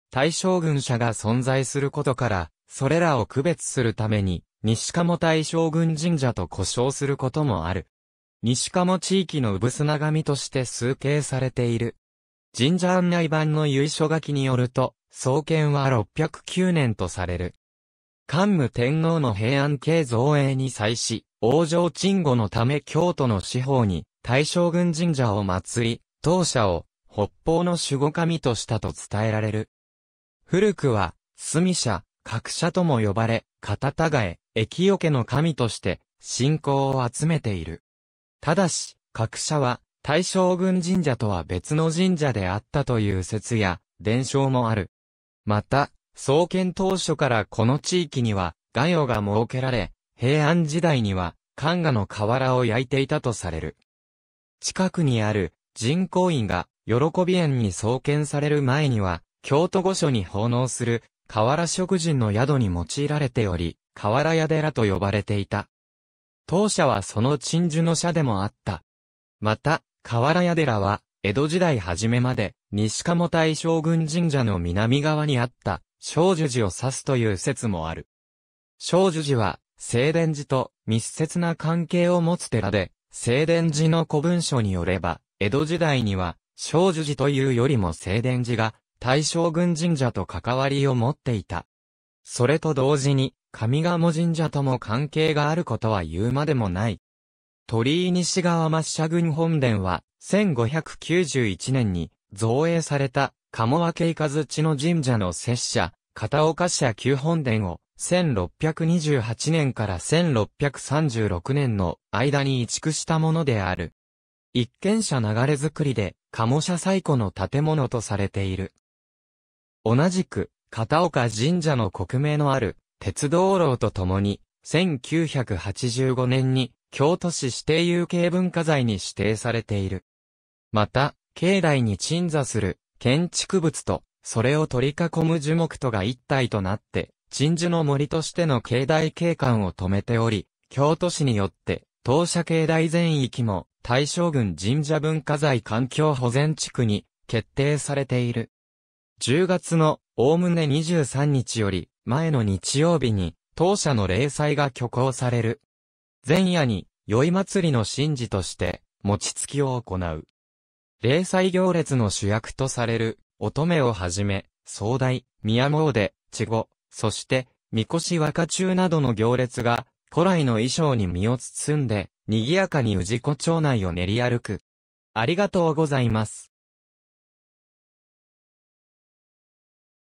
大将軍神社 (京都市北区)
大将軍神社（たいしょうぐんじんじゃ）は、京都市北区西賀茂角社町にある神社。
京都市内に複数の「大将軍神社、大将軍社」が存在することから、それらを区別するために西賀茂大将軍神社（にしがもたいしょうぐんじんじゃ）と呼称することもある。
西賀茂地域（今原、鎮守庵、総門、田尻）の産土神（氏神）として崇敬されている。
神社案内板の由緒書きによると、創建は609年（推古17年）とされる。
桓武天皇の平安京造営に際し、王城鎮護のため京都の四方に「大将軍神社」を祀り、当社を北方の守護神としたと伝えられる。
古くは「須美社」「角社」（すみやしろ）とも呼ばれ、方違え（かたたがえ、かたちがえ）、疫除けの神として信仰を集めている。
ただし、「角社」は大将軍神社とは別の神社であったという説や伝承もある。
また、創建当初からこの地域には瓦窯が設けられ、平安時代には官衙（かんが、古代の役所）の瓦を焼いていたとされる。
近くにある神光院が慶円に創建される前には、京都御所に奉納する瓦職人の宿に用いられており、「瓦屋寺」と呼ばれていた。
当社はその鎮守の社でもあった。
また「瓦屋寺」は江戸時代初めまで西賀茂大将軍神社の南側にあった正受寺をさすという説もある。
正受寺は正伝寺と密接な関係を持つ寺で、正伝寺の古文書（弘化年間の寺の日記）によれば、江戸時代には正受寺というよりも正伝寺が大将軍神社と関わりを持っていた。
それと同時に上賀茂神社とも関係があることは言うまでもない。
鳥居 西側末社群 本殿は、1591年（天正19年）に造営された賀茂別雷神社（上賀茂神社）の摂社・片岡社旧本殿を、1628年（寛永5年）から1636年（寛永13年）の間に移築したものである。
一間社流造（いっけんしゃながれづくり）で、賀茂社最古の建物とされている。
同じく片岡神社の刻銘のある鉄燈籠（とうろう）とともに、1985年（昭和60年）に京都市指定有形文化財に指定されている。
また、境内に鎮座する建築物と、それを取り囲む樹木とが一体となって、鎮守の森としての境内景観を留めており、京都市によって当社境内全域も「大将軍神社文化財環境保全地区」に決定されている。
10月の、概ね23日より前の日曜日に、当社の例祭が挙行される。
前夜に宵祭の神事として餅搗（もちつき）を行う。
例祭行列の主役とされる「乙女」（主に小学校高学年の女子七名が務める）を始め、「総代」（氏子各町会長）、「宮詣」（乙女の警護を担う男性）、「稚児」（主に就学前児童）、そして「神輿若中」（主に小学生）などの行列が、古来の衣装に身を包んで、賑やかに氏子町内を練り歩く。